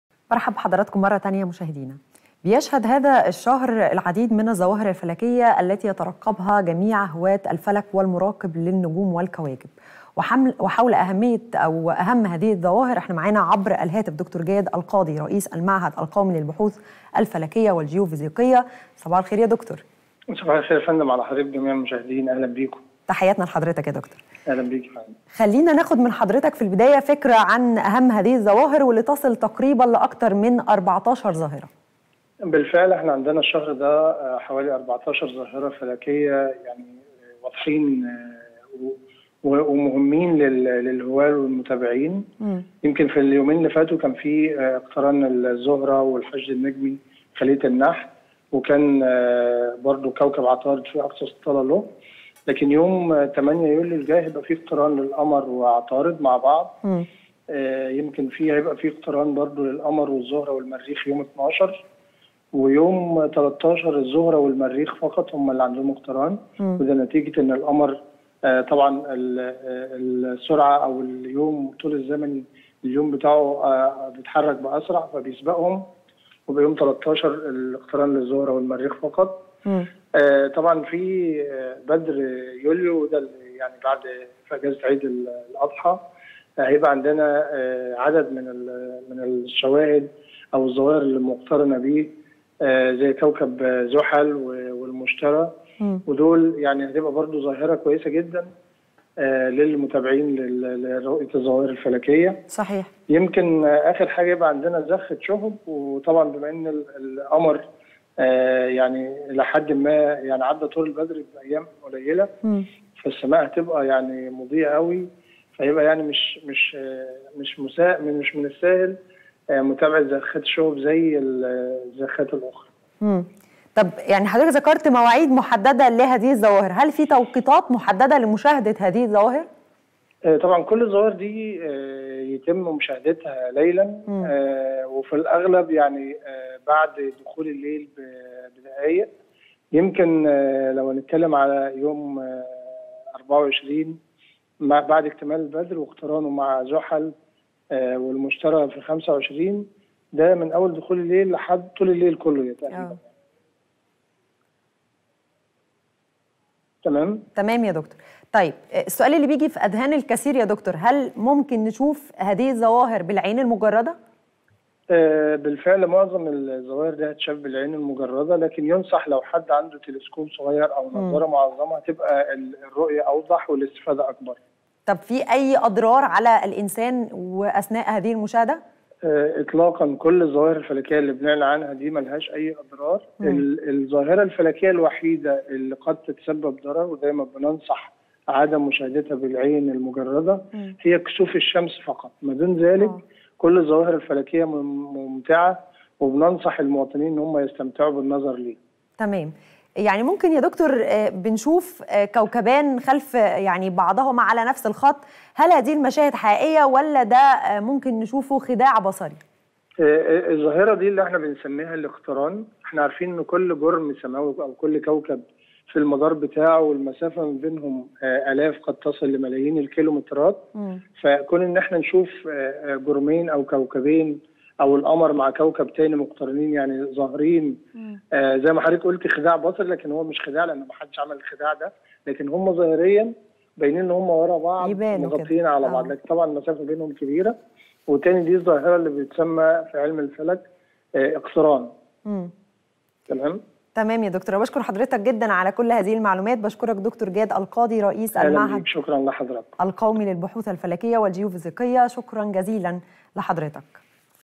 14 ظاهرة فلكية هذا الشهر يمكنك رؤيتها ليلًا! -د/ جاد القاضي يكشف الأمر
14 ظاهرة فلكية هذا الشهر يمكنك رؤيتها ليلًا! -د/ جاد القاضي يكشف الأمر